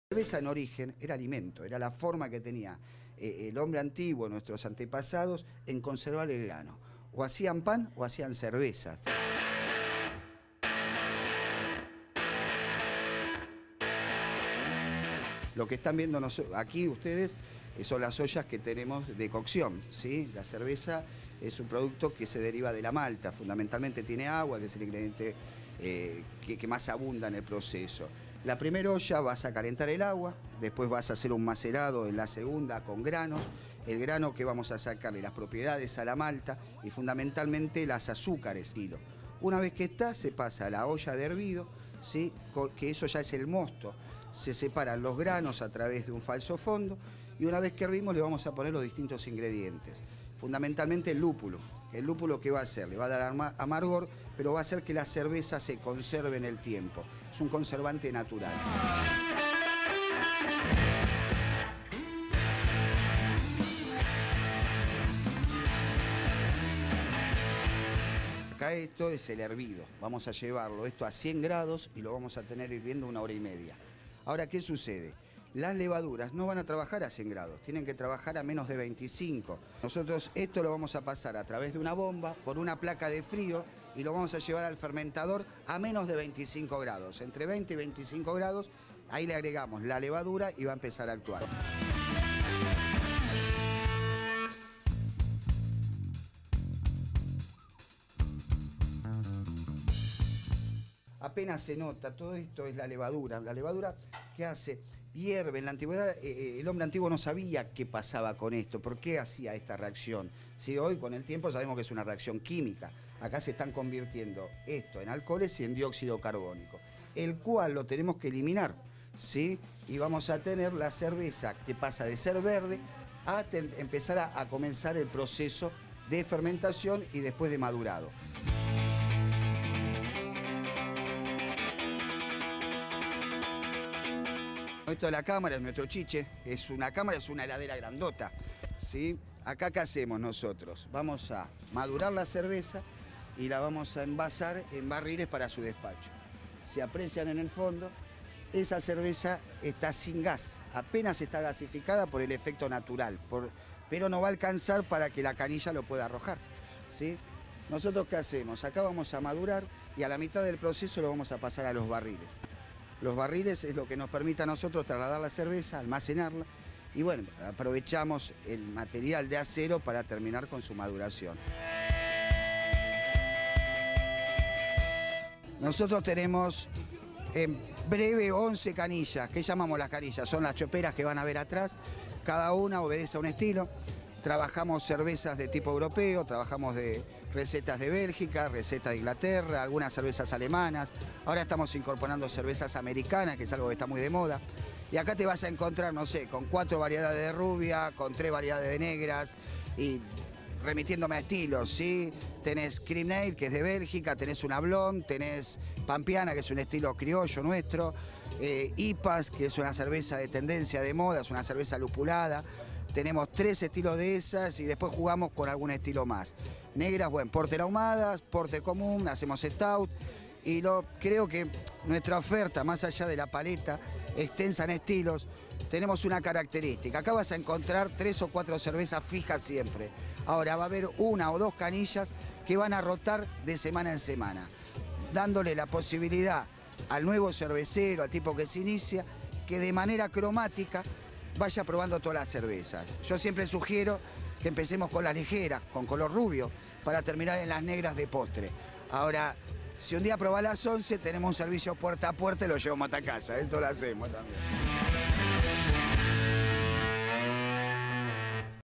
CERVEZA ARTESANAL, Y LOCAL
Cada vez son más los emprendedores que se dedican a elaborar su propia cerveza de manera artesanal y casera. Hoy conocemos a Pablo Balor, un vecino de Villa Ballester que nos cuenta de qué se trata este mundo y cómo se logra una buena cerveza.